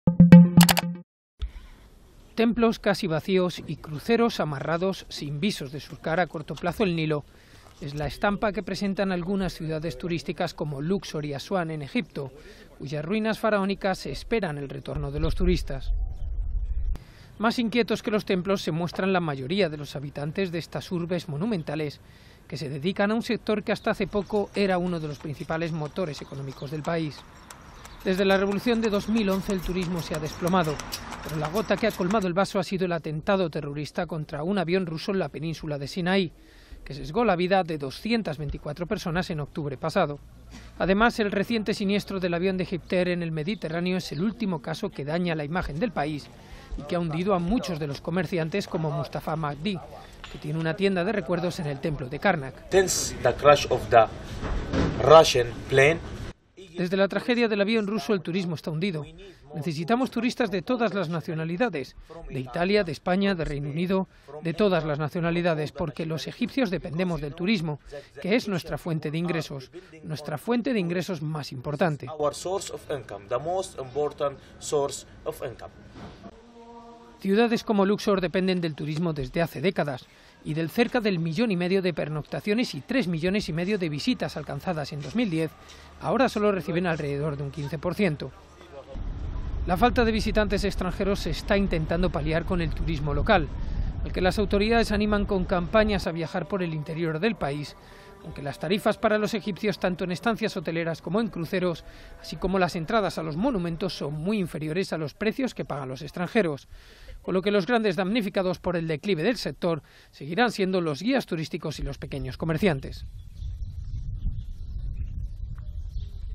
Egipto espera en letargo el regreso de los turistas
Luxor (Egipto), 30 may (EFE).- (Imagen:Marina Villén) Templos casi vacíos y cruceros amarrados sin visos de surcar a corto plazo el Nilo es la estampa que presentan las ciudades de Luxor y Asuán, cuyas ruinas faraónicas esperan impertérritas el retorno de los turistas.